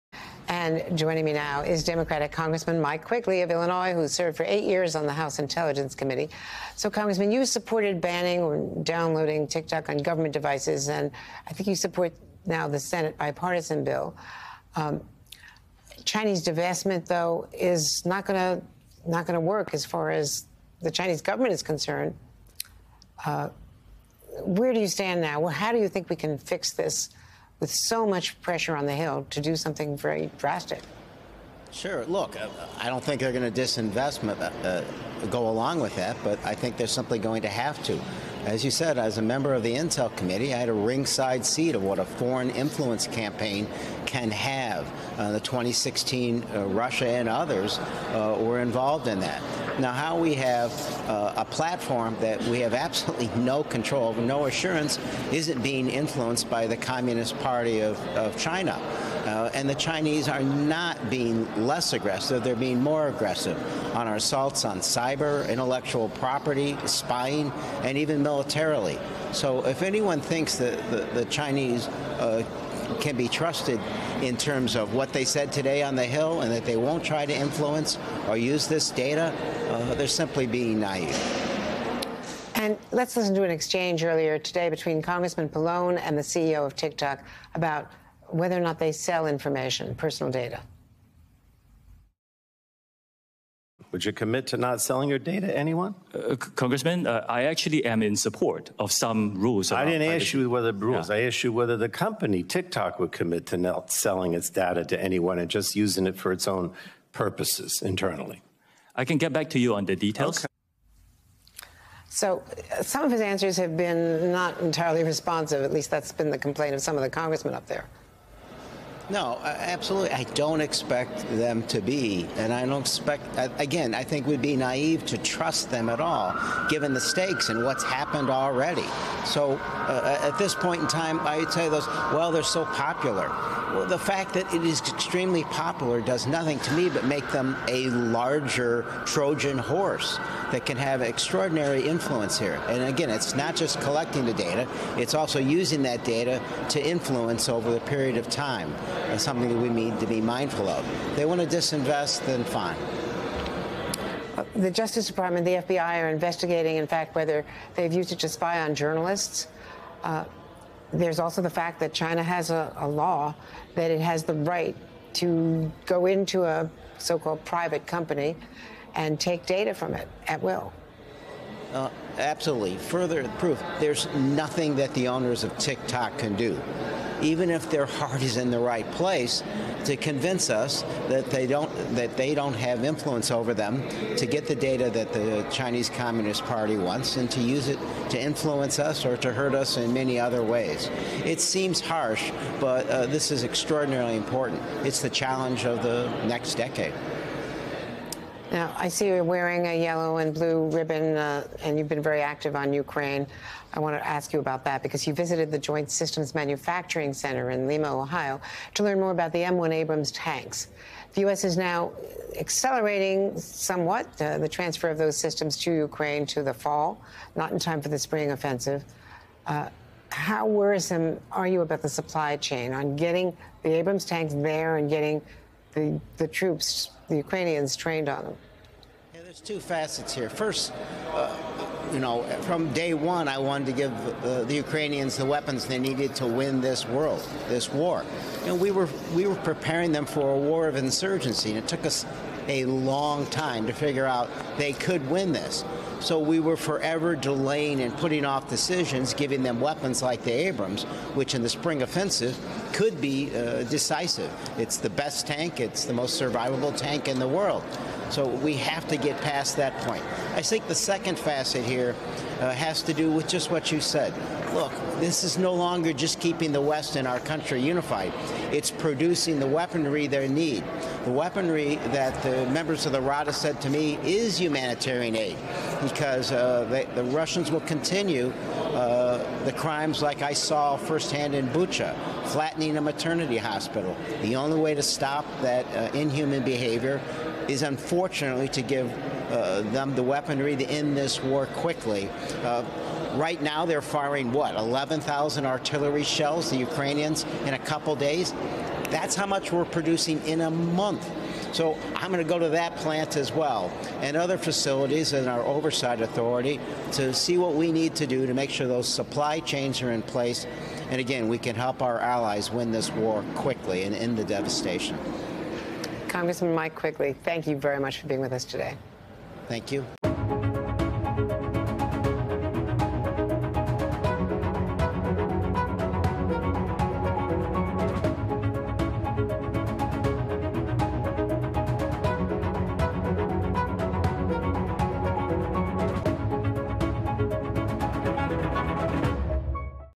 Rep, Quigley: TikTok growing in popularity just gives China ‘a larger Trojan horse’ to spy on U.S.
Representative Mike Quigley (D-IL) joins Andrea Mitchell to react to the hearing on TikTok by the House Energy and Commerce Committee, where the social media giant’s CEO repeatedly assured members that consumer data is safe on the app. “We have a platform that we have absolutely no control of, no assurance of if it’s being influenced by the Communist Party of China, and the Chinese are not being less aggressive. They're being more aggressive on our assaults on cyber intellectual property, spying, and even militarily,” says Quigley. “The fact that it is extremely popular does nothing to me but make them a larger Trojan horse that can have extraordinary influence here. And again, it's not just collecting the data, it's also using that data to influence over a period of time and that’s something that we need to be mindful of.”